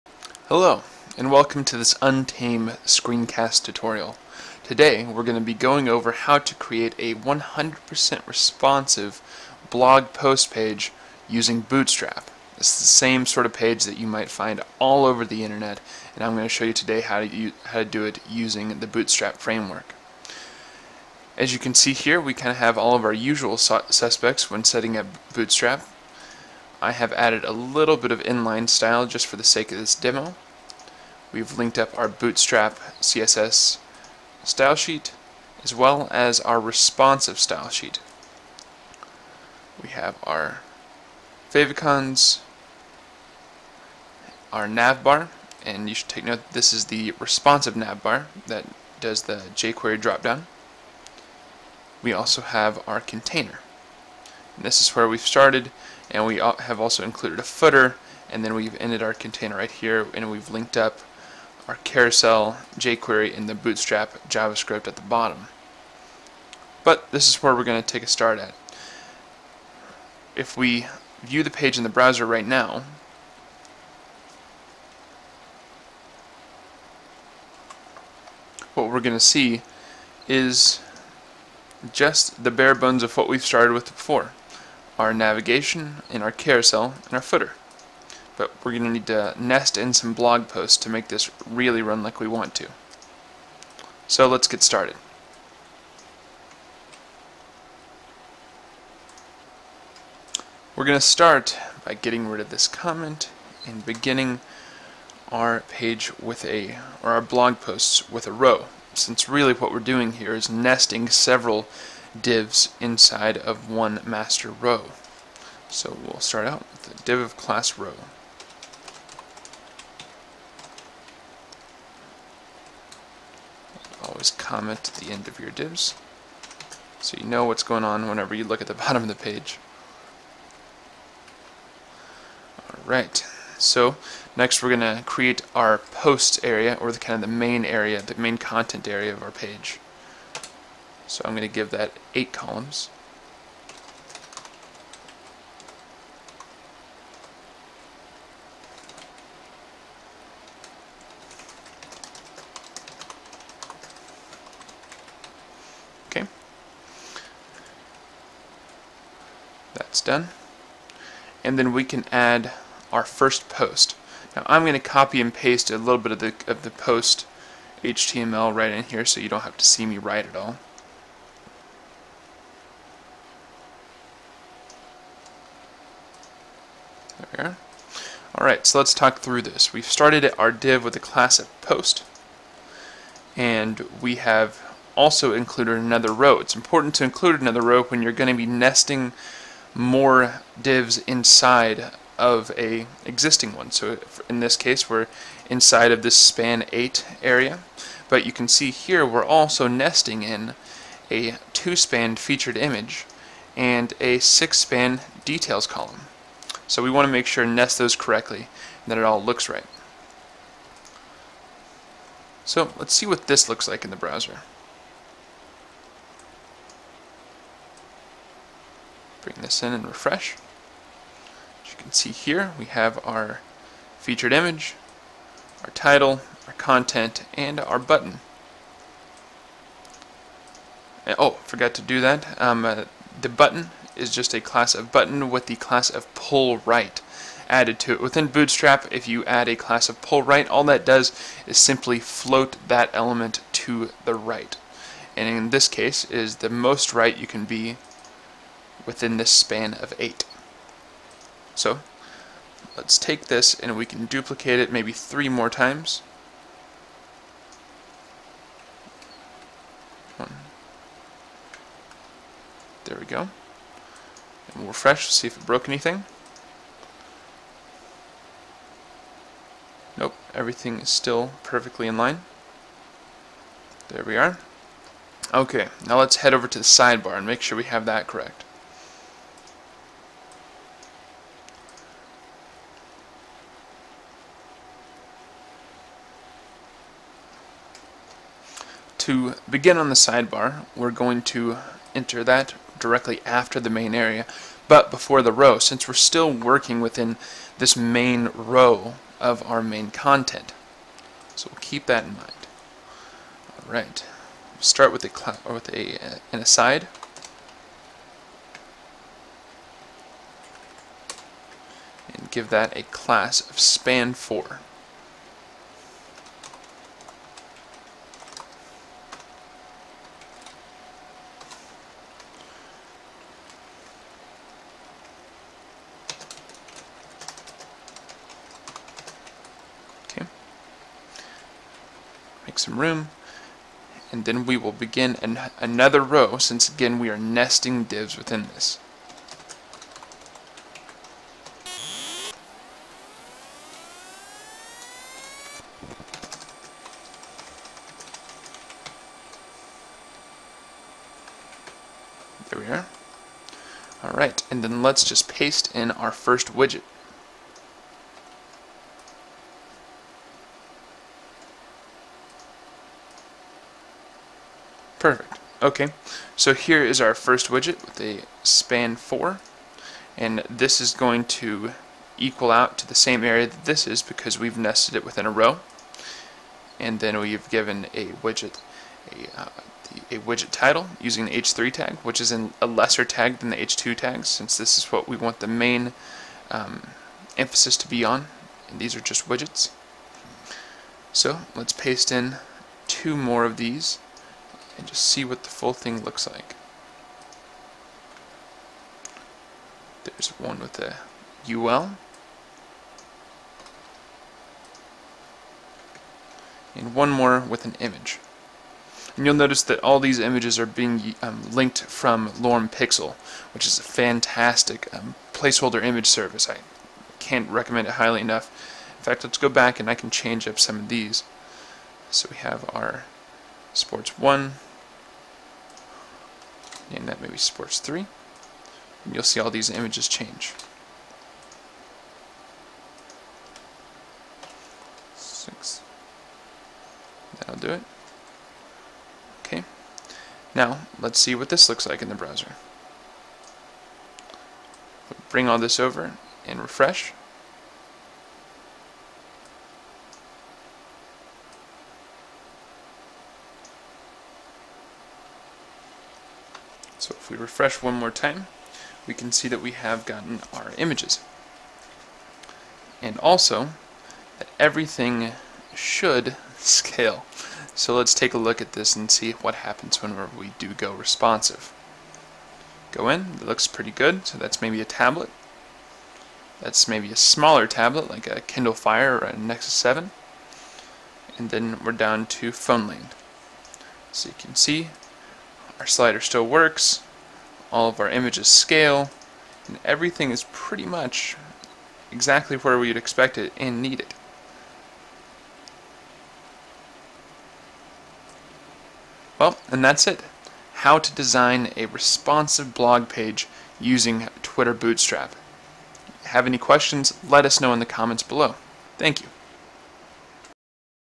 Create A Responsive Blog Using Twitter Bootstrap
Create a Responsive Blog in under 10 minutes using Twitter Bootstrap